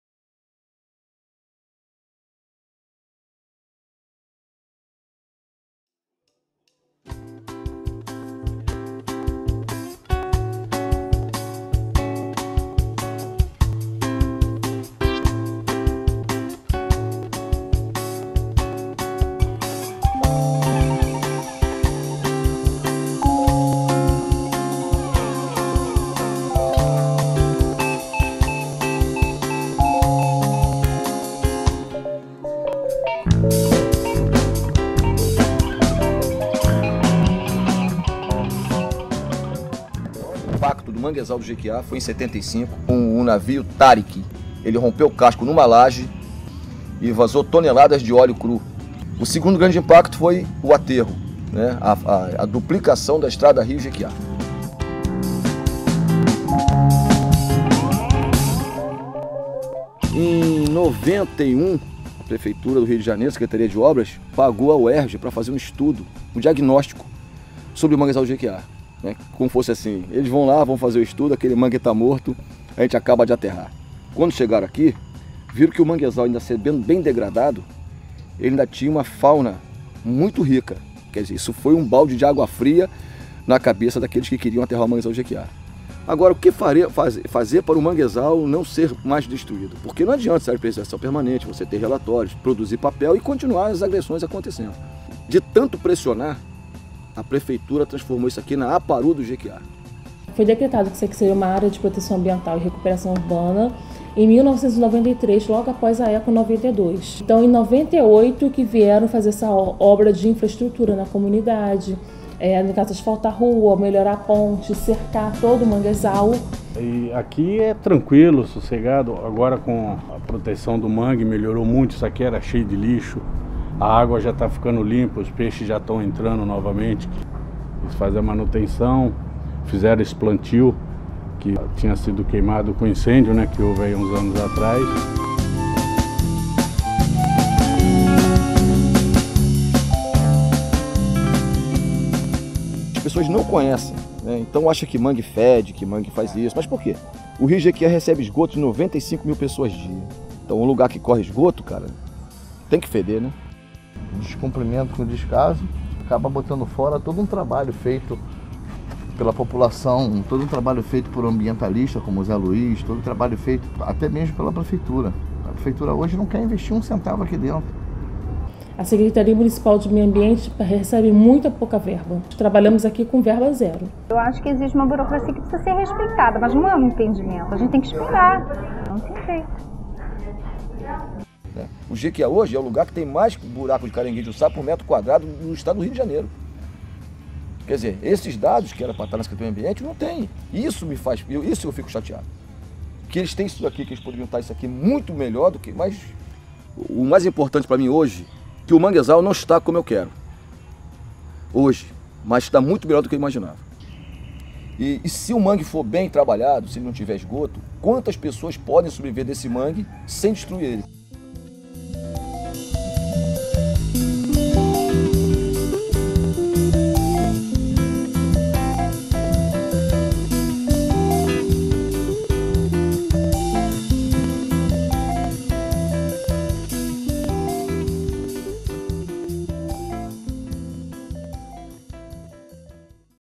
Manguezal do Jequiá
O manguezal mais preservado do Rio de Janeiro  merece mais atenção do governo do Estado para continuar a garantir a sobrevivência da fauna e das comunidades de pescadores